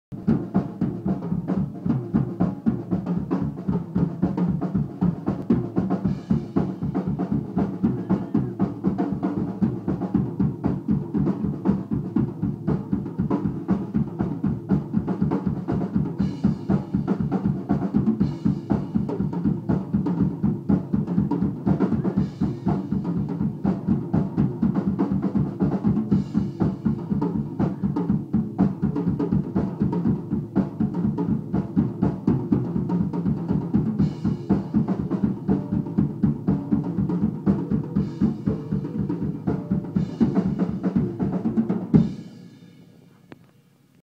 Polly tribal dancing!!
Polly going into a rhythmic trance with pet chimp!!